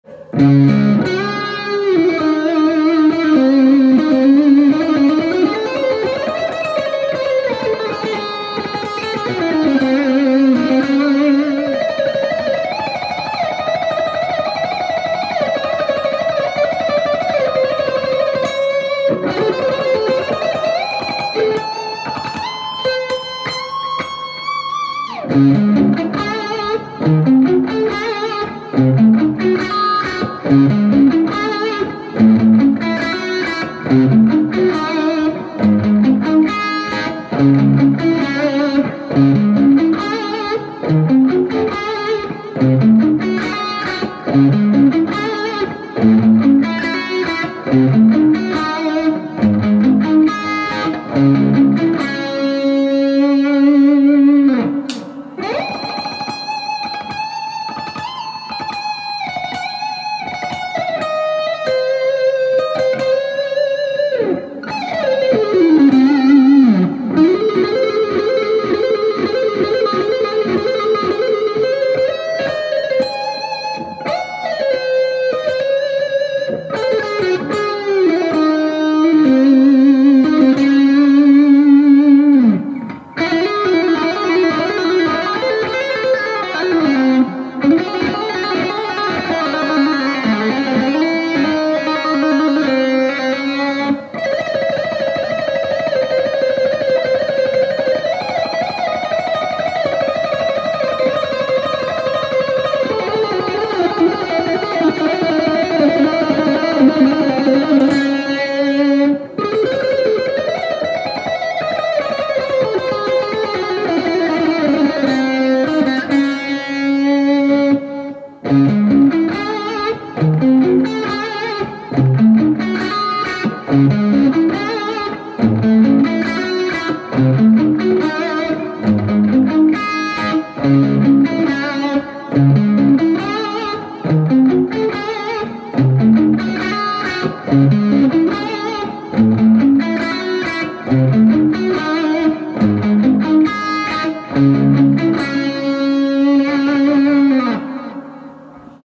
Melodic Neoclassical Shred Guitar Solo In Dmi...Yngwie, Ritchie Blackmore, Uli Jon Roth Style!
Neoclassical melodic shred guitar solo in the style of Yngwie Malmsteen, Ritchie Blackmore, Vinnie Moore, David T Chastain,  and Paul Gilbert on a 1985 Aria Pro ll guitar through a Positive Grid Spark 40watt amp.